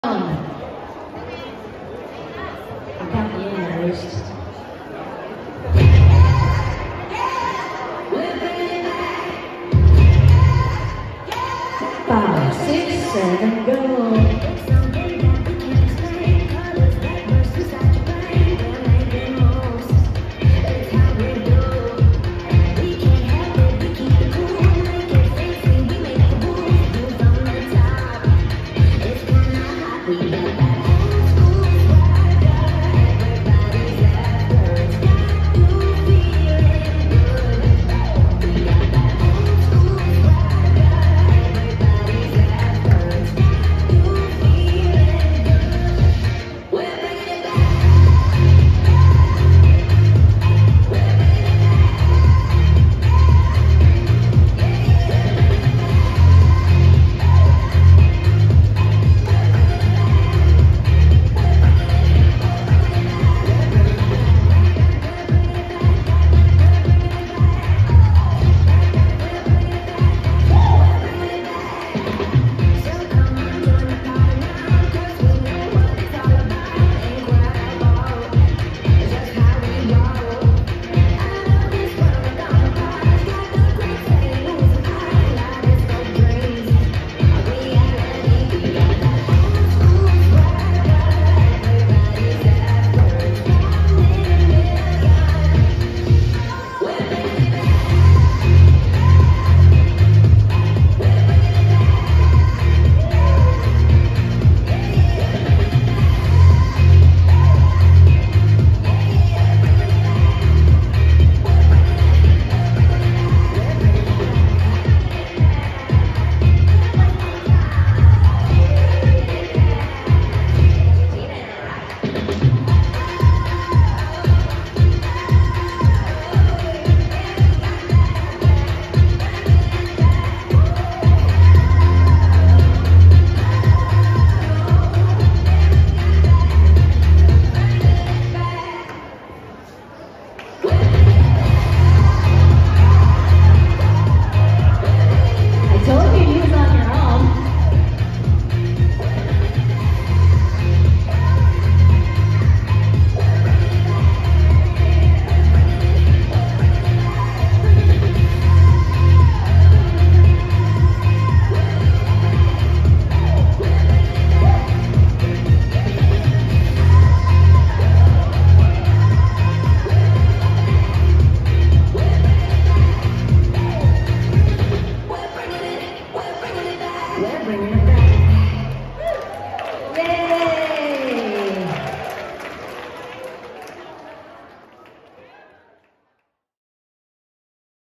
Bringing It Back - PSWB Line Dance Event 2024
PSWB Palm Springs Winter Break - Line Dance Event 2024
Dance: Bringing It Back
Choreographer: Fred Whitehouse (IRE) - December 2023